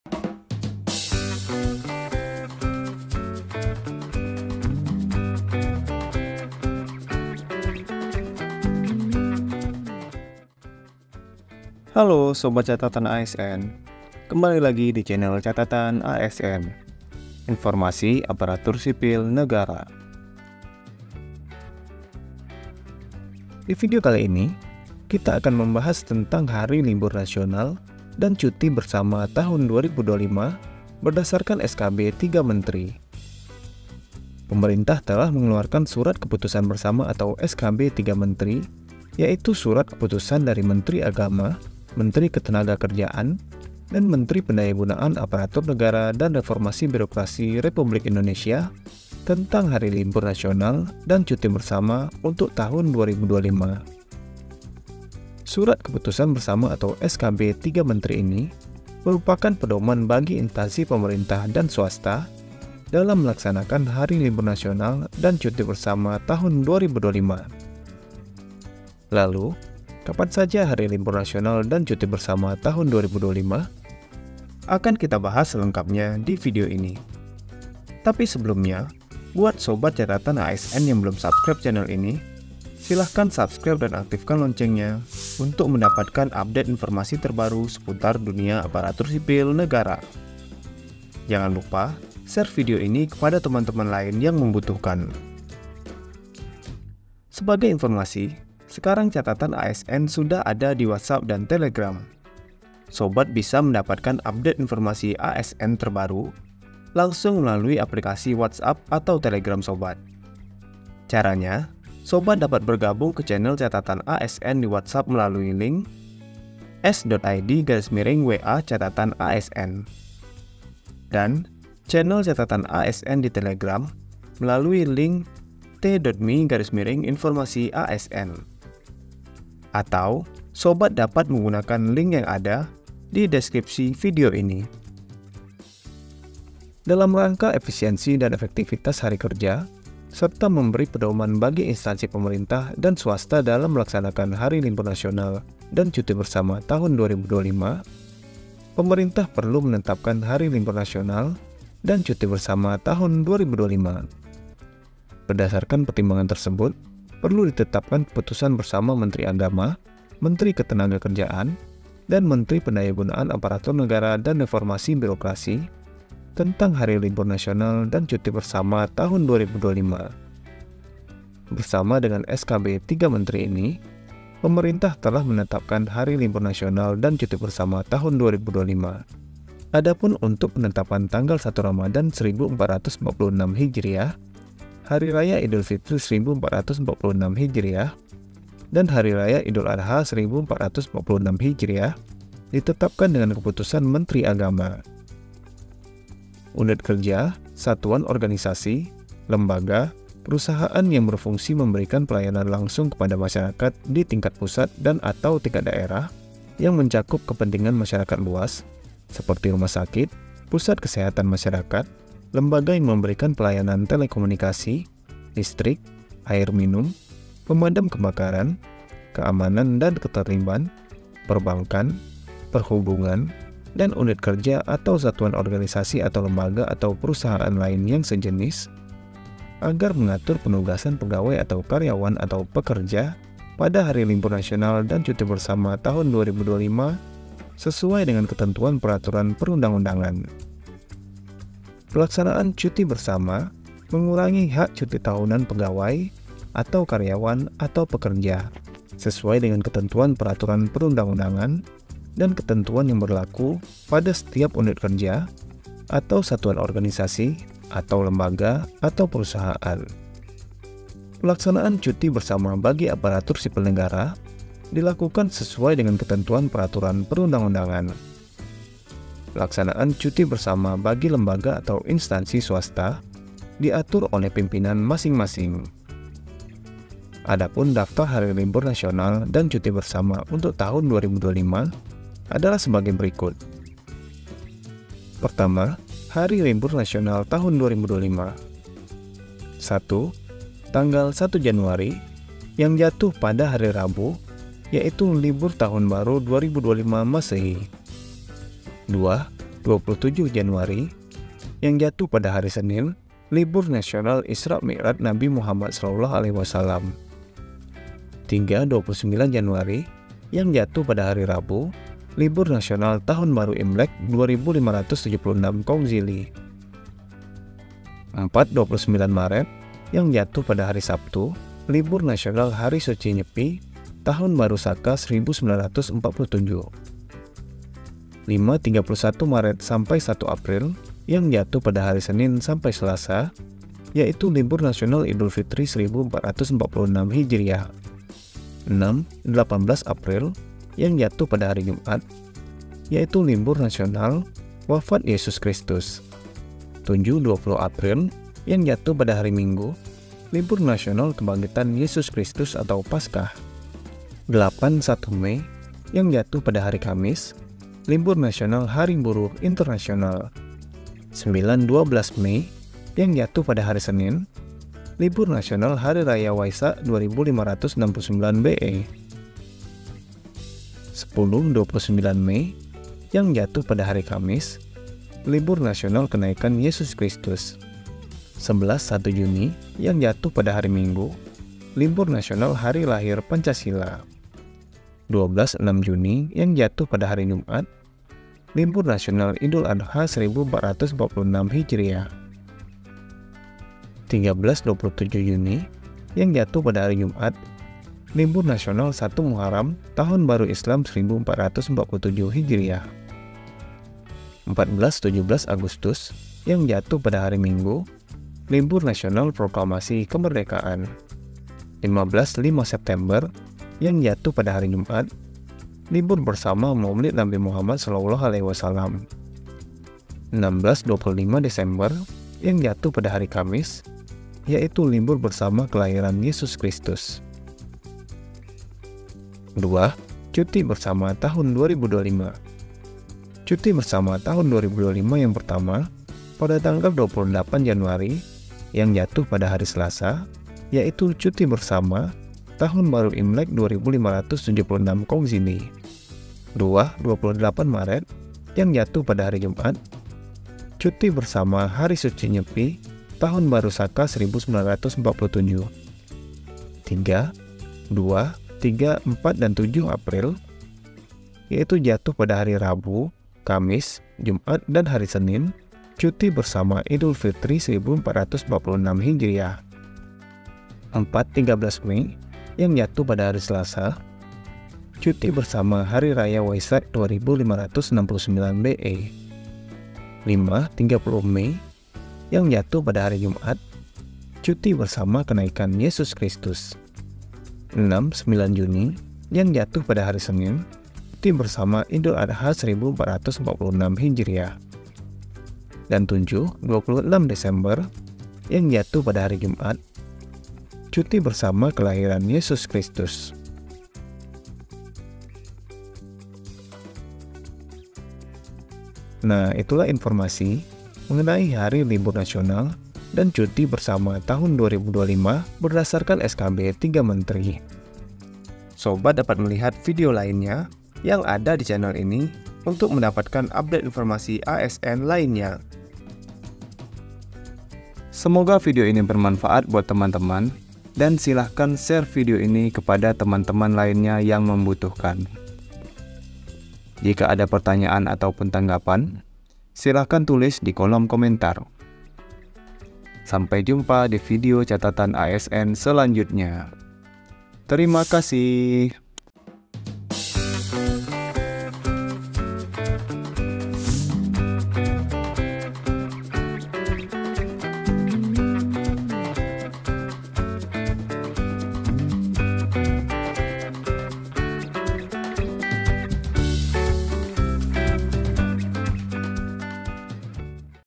Daftar Hari Libur Nasional dan Cuti Bersama Tahun 2025 – SKB 3 Menteri Libur Nasional 2025
Pemerintah telah mengeluarkan Surat Keputusan Bersama 3 Menteri yaitu Menteri Agama, Menteri Ketenagakerjaan dan Menteri Pendayagunaan Aparatur Negara dan Reformasi Birokrasi Republik Indonesia tentang Hari Libur Nasional dan Cuti Bersama Tahun 2025

Peraturan ini merupakan pedoman bagi Instansi Pemerintah dan Swasta dalam melaksanakan hari libur nasional dan cuti bersama tahun 2025.

Lalu, Kapan saja hari libur nasional dan cuti bersama tahun 2025?

Akan Kita Bahas Selengkapnya di video ini. Jadi, simak video ini sampai selesai ya 😉

Silahkan share video ini ke teman-teman lain yang membutuhkan 💖